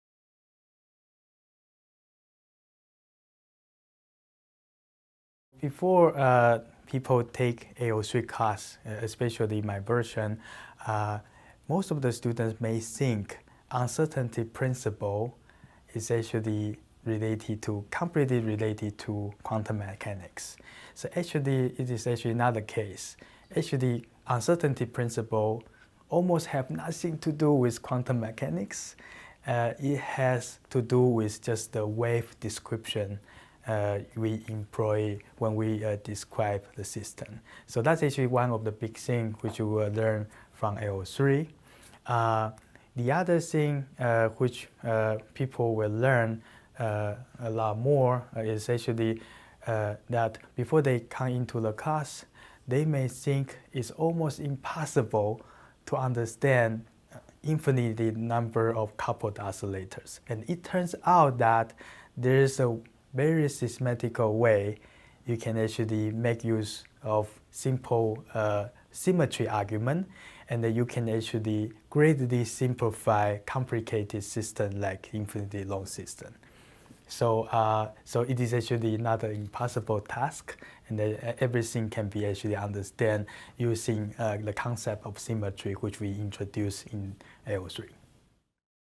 Students' Common Misconceptions
MIT 8.03SC Physics III: Vibrations and Waves, Fall 2016
Instructor: Yen-Jie Lee

In this video, Yen-Jie Lee shares common misconceptions about physics that students bring to the course.